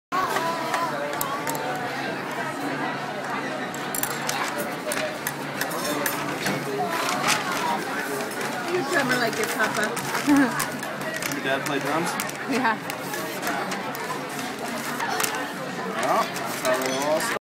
Drummer Girl
Using her chopsticks at P.F. Changs